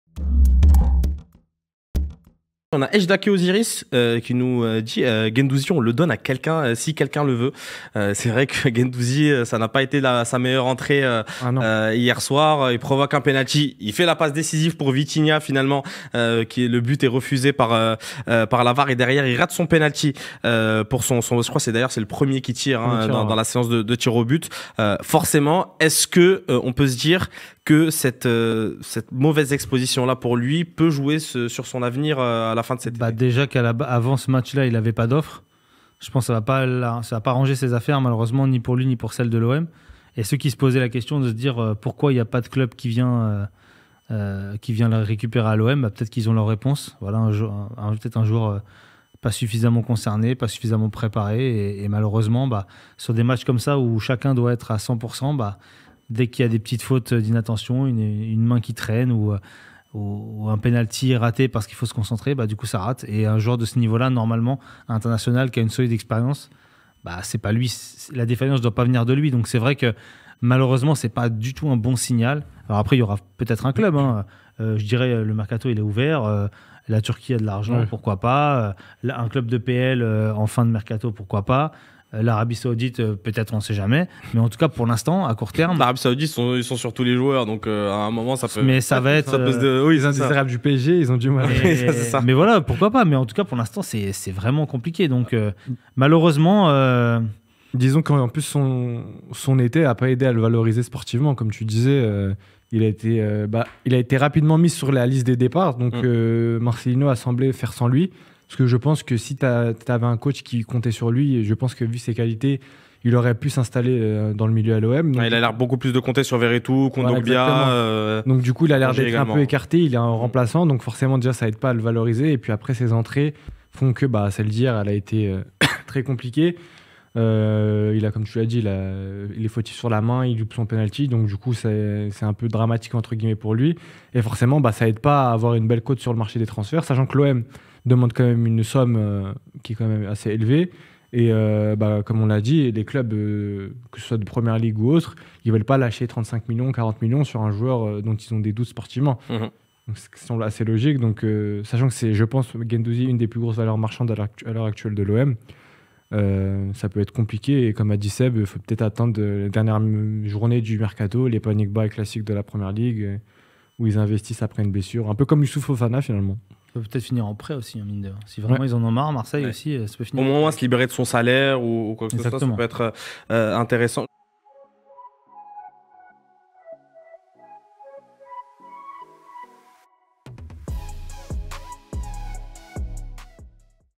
L'OM ne sait PLUS QUOI FAIRE avec Mattéo Guendouzi !
Mattéo Guendouzi devrait payer l'embouteillage au milieu de terrain cette saison à l’OM. L’international français, qui a vécu une soirée compliquée lors du barrage perdu contre le Panathinaïkós, est sur la liste des départs.

Nos 3 journalistes évoquent les points de chute possibles.